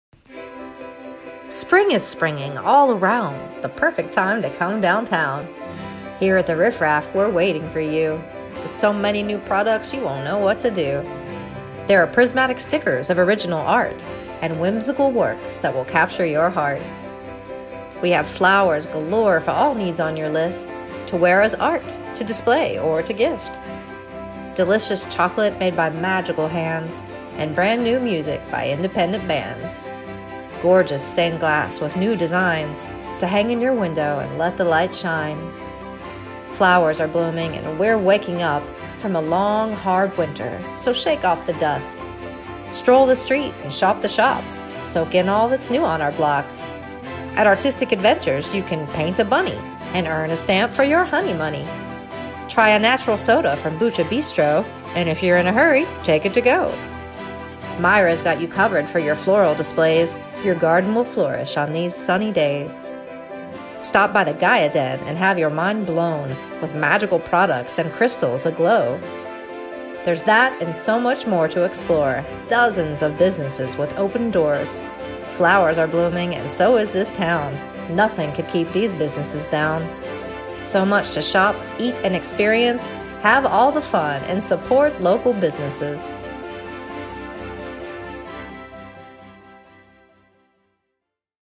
Spring is Springing!
Springtime is springing! Check out out that's *NEW* at The RiffRaff Art Boutique & around our blossoming neighborhood, the Mercer Street Grassroots District in downtown Princeton, West Virginia.